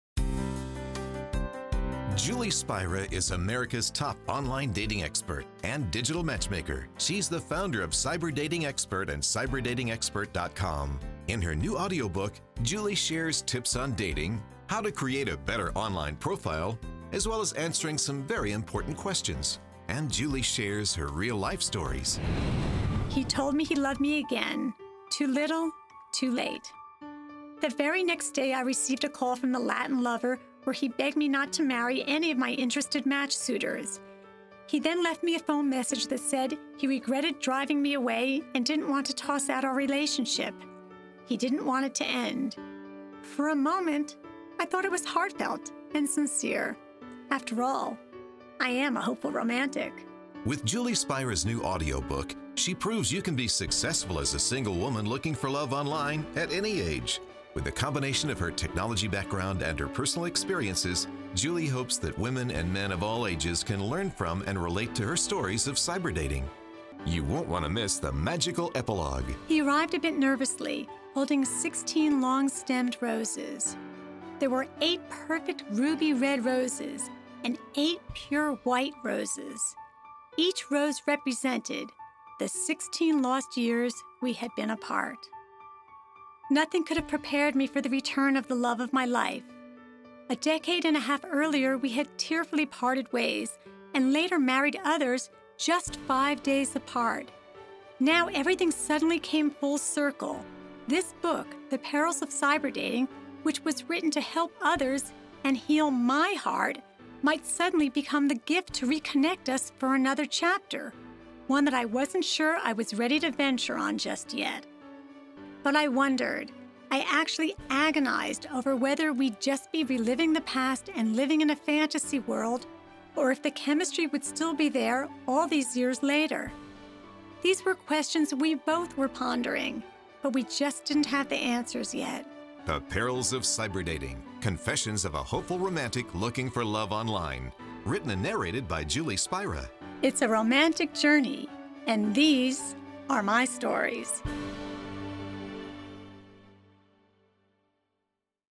Julie Spira Book Trailer - The Perils of Cyber-Dating
Online Dating Expert Julie Spira narrates her Internet Dating book, "The Perils of Cyber-Dating: Confessions of a Hopeful Romantic Looking for Love Online," with new bonus dating advice and a new magical epilogue, guaranteed to help you believe in love.